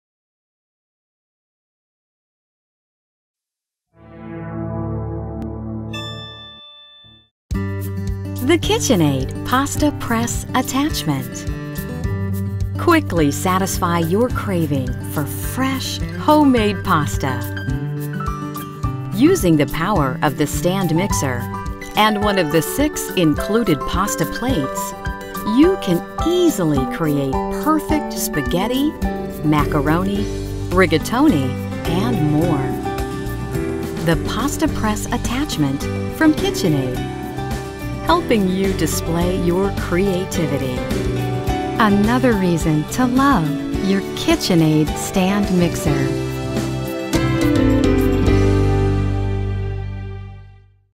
Tvarovač těstovin KitchenAid KPEXTA
Příslušenství pro kuchyňský robot Kitchen Aid - Tvarovač italských těstovin KPEXTA.